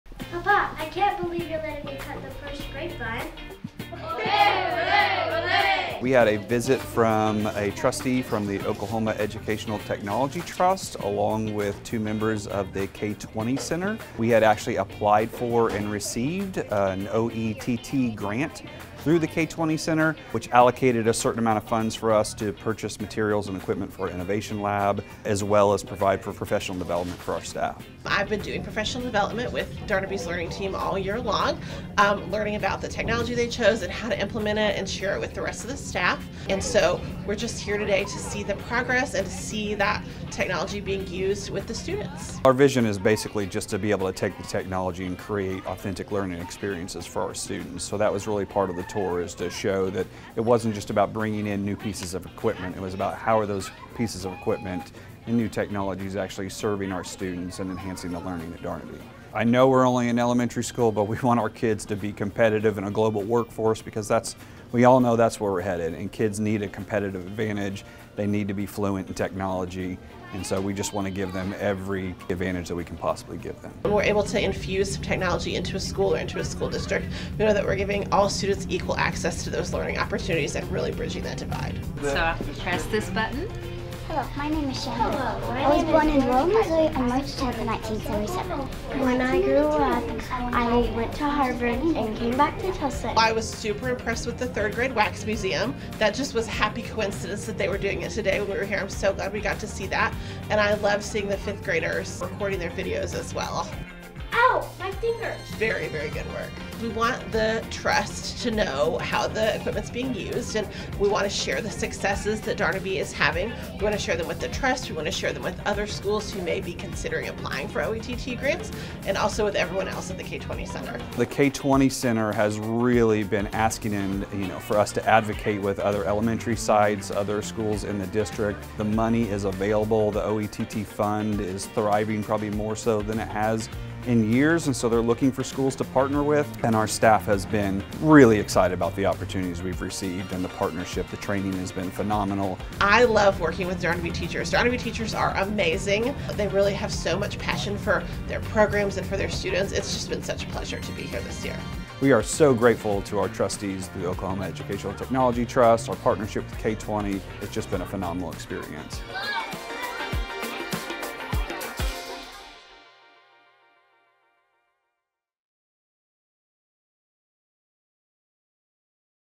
Darnaby Elementary Hosts OETT Representatives, Encourages Other Schools to Apply for Grants
Representatives from the Oklahoma Educational Technology Trust and the University of Oklahoma’s K20 Center toured Darnaby Elementary School to see how students and staff are using technology that was purchased through their grant. The K20 Center has also been collaborating with Darnaby teachers on professional development this year and are actively looking for more schools to partner with.

March 2023